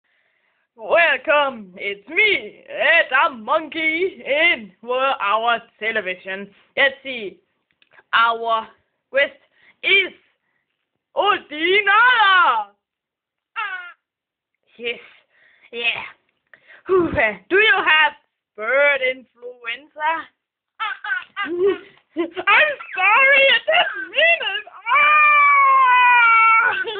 Monkey Quiz
a funny monkey hwos name is adam, and his friend Oldin-Odder Adam is the man for the Quiz